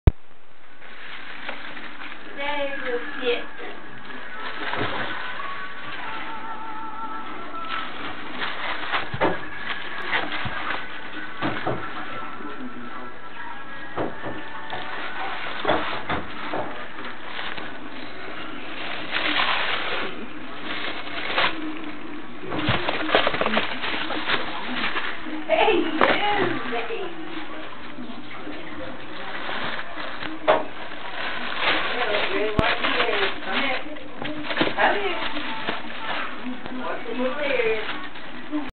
Nanoki and Seiko wardancing and mock-fighting!
This is just a short vid of 2 of my ferrets, Nanoki and Seiko playing by the ferret gate. They're kinda attacking eachother and the tissue paper. ^.^

(Don't mind the cackle towards the end if you can hear it..my mom was being silly!)